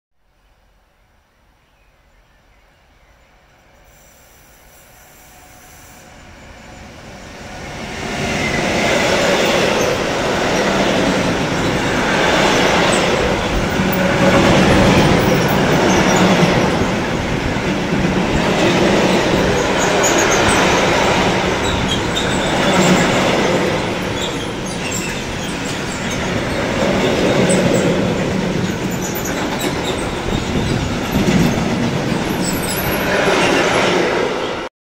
Eine Br 85 120-3 mit einem langen Güterzug
Schuldigung dass das Viedeeo teilweise wackelig und unscharf ist